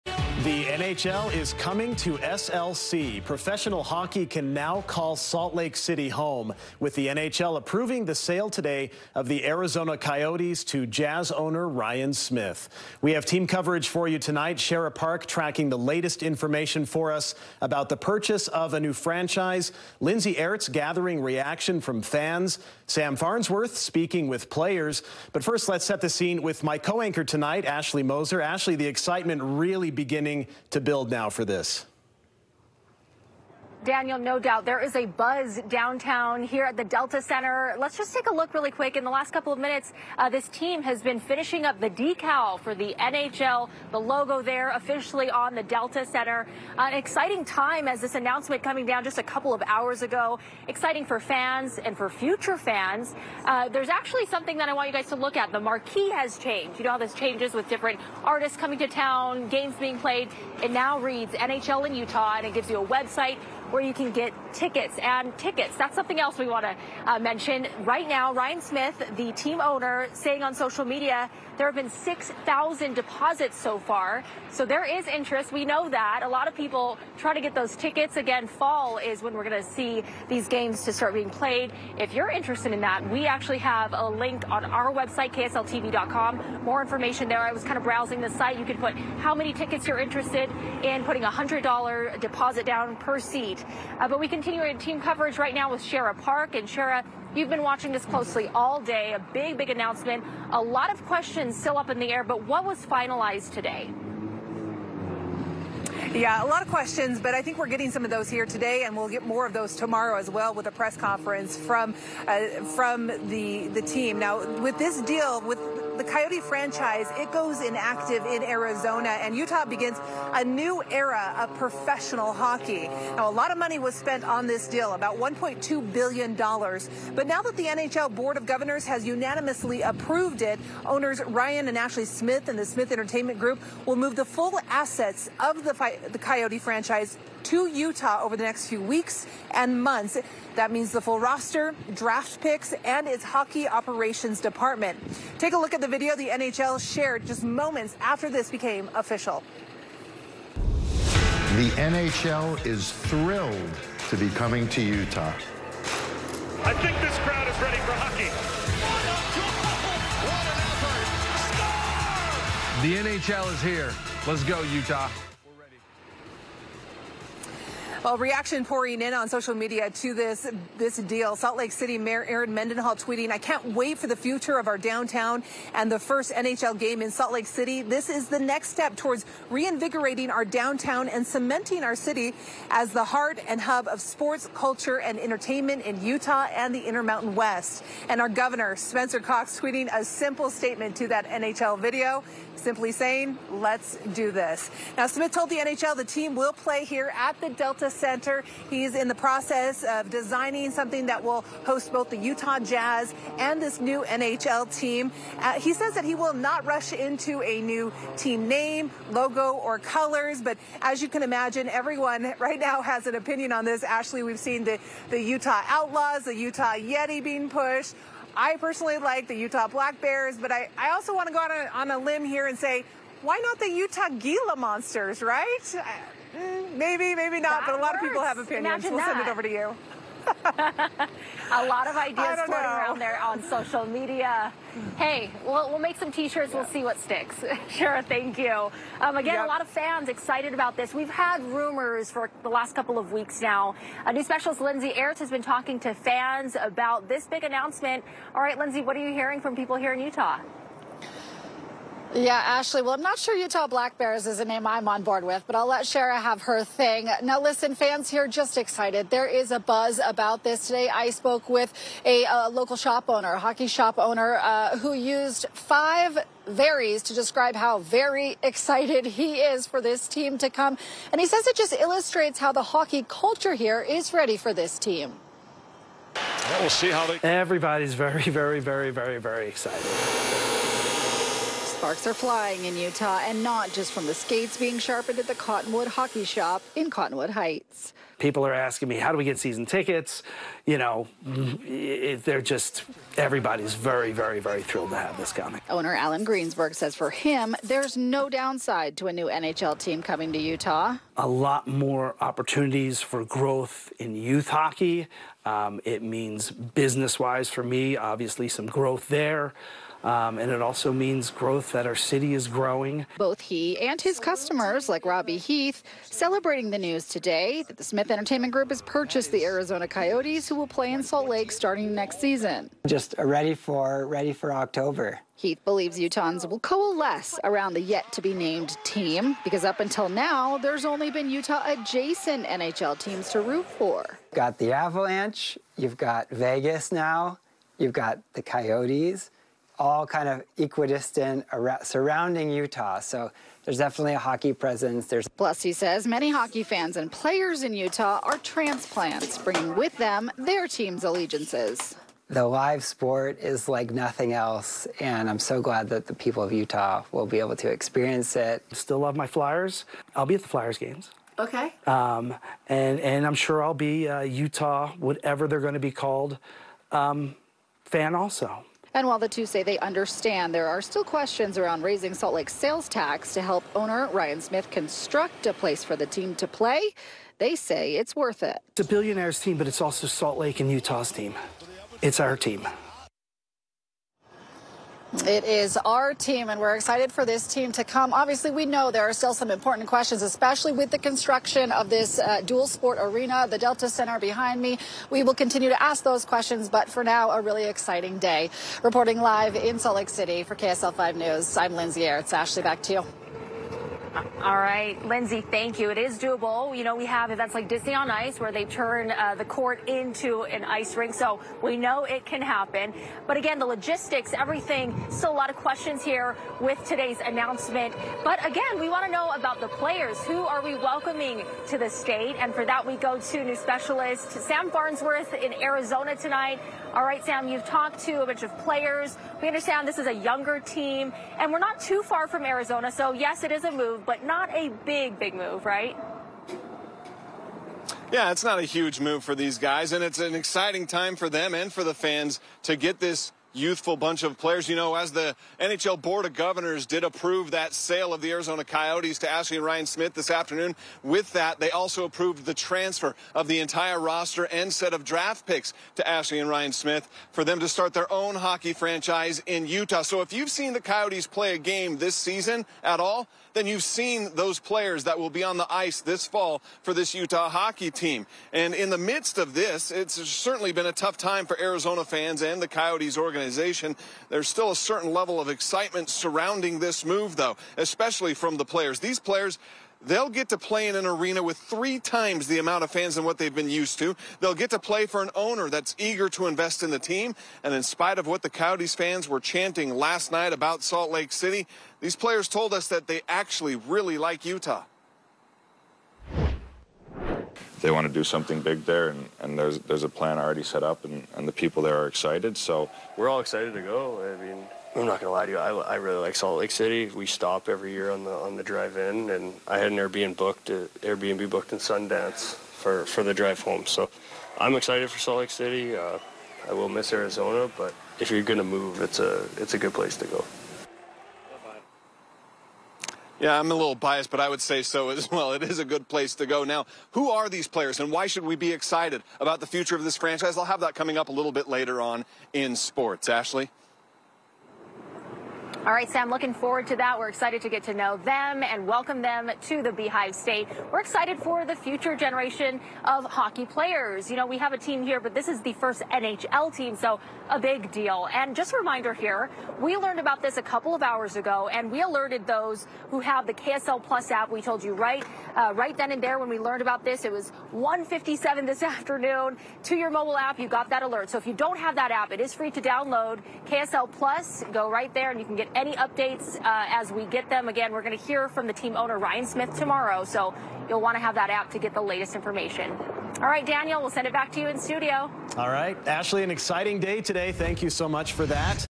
League approves new NHL team in Utah owned by Ryan, Ashley Smith
The NHL Board Of Governors unanimously approved the establishment of a new hockey franchise in Utah by Ryan Smith.